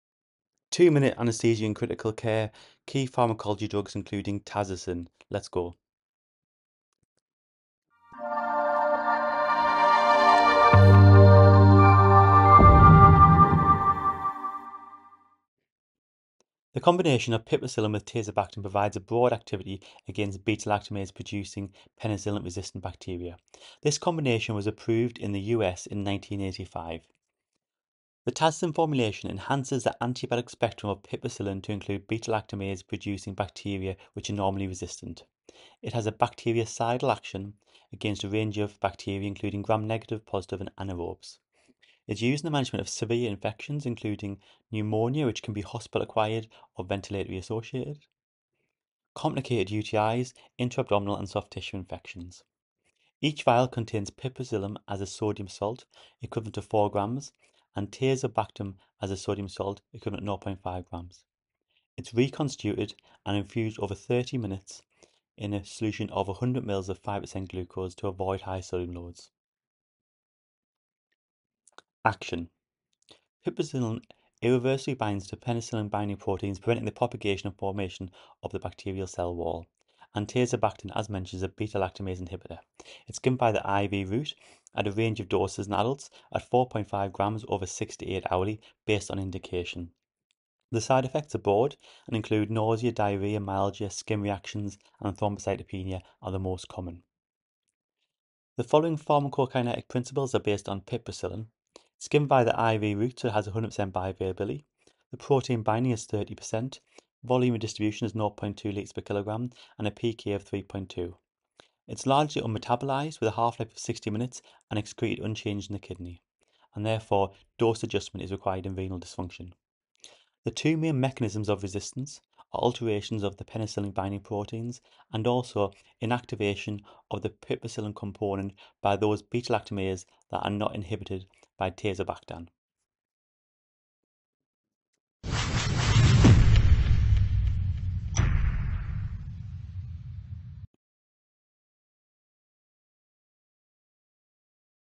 What you SHOULD know about TAZOCIN (Mechanism of action)
Piperacillin with tazobactam
Key pharmacology
NO CLINICAL ADVICE GIVE
Indications, use, mechanism of action, pharmacokinetics